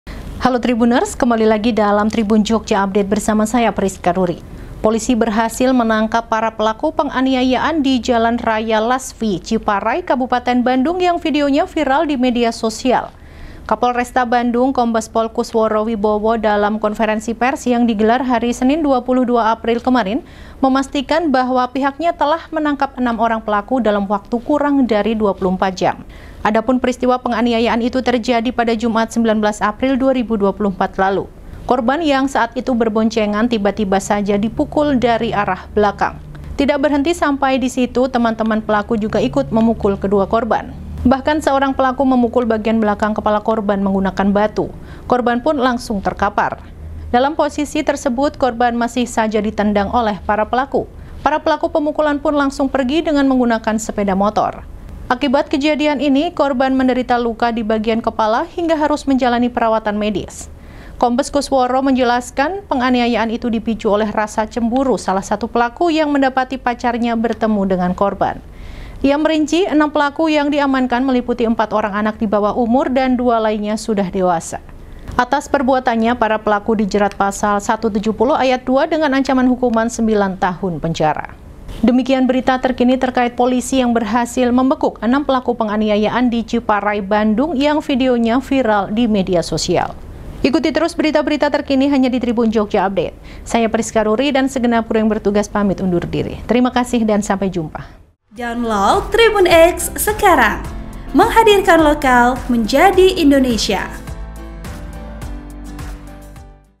POLISI BEKUK 6 PELAKU PENGANIAYAAN YANG VIDEONYA VIRAL DI MEDSOS
Kapolresta Bandung Kombes Pol Kusworo Wibowo dalam konferensi pers yang digelar hari Senin 22 April 2024 kemarin memastikan bahwa pihaknya telah menangkap enam orang pelaku dalam waktu kurang dari 24 jam.

Adapun peristiwa penganiayaan itu terjadi pada Jumat, 19 April 2024 lalu.

Editor Video : Veri Vesiano
Narator : Prisca Ruri
Editor Naskah : Mona Kriesdinar
Produser : Ribut Raharjo
(vsn)